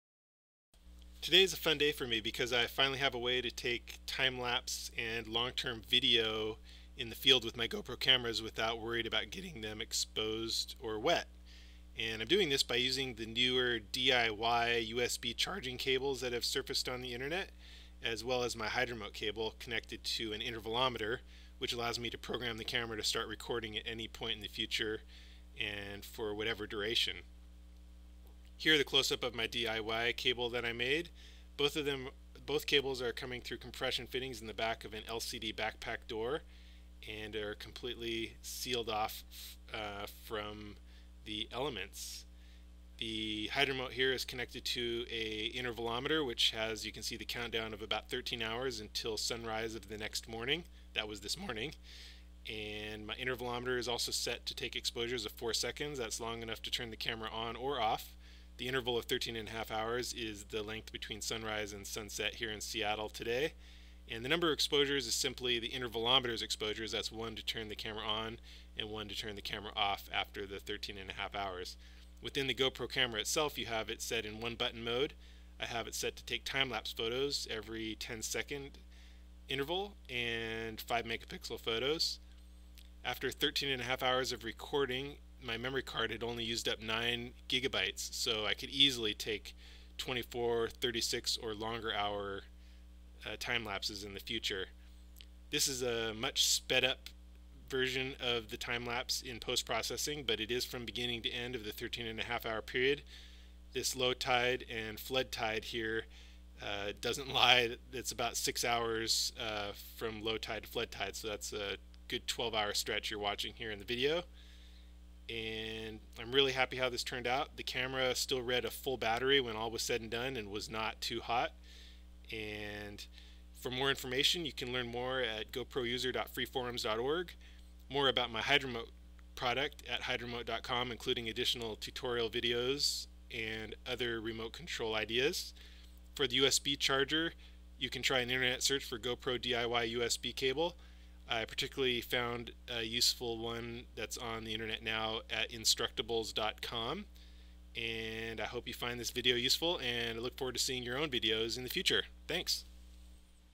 Timer Programmable Long Term Timelapse for GoPro - waterproof
Here's a solution for permanently charging your GoPro while sealed inside its waterproof housing... and program it to start and stop recording hours or days in the future!  Recently, a couple innovators have shown how to modify USB mini cables to fit inside the housing (links are below).  When coupled with a HydRemote Cable System and intervalometer, you can program your Gopro HD Hero , Hero2, or Hero3 White to record video or time lapses for many more hours than the typical 2 hour battery life of your camera (camera must be in 'one button mode').  The GoPro can remain dormant for days or weeks, too, which makes this a great option for remote unmanned locations.

In this demo, I programmed the intervalometer to record a timelapse the next day from sunrise to sunset, 10 second interval 5 megapixel photos.  The system worked flawlessly, and after 13.5 hours of recording, I had only recorded 9 gigabytes of data.  I used a 5m usb cable plugged into an outlet, but an external battery source would have worked just as well.  After recording ended, my Hero2 still registered a completely full battery.  I sped up the timelapse in post-processing, but it is uncut from beginning to end of the 13.5 hour recording.

About the timelapse:  This is a southward looking view of Puget Sound from Burien, Washington, USA, showing the 6 hour ebb and flood tides of the day, sunrise to sunset.  March 2, 2013.